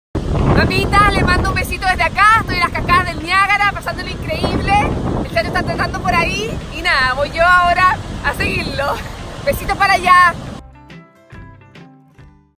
SOS MAMIS - VACACIONES CON TRINI - CATARATAS
Mamitas, estas vacacionescontrini están siendo salvaje ❤️ tomando un bañito en las cataratas del Niagara 🙈 y persiguiendo al Chancho mientras trota 🏃⁠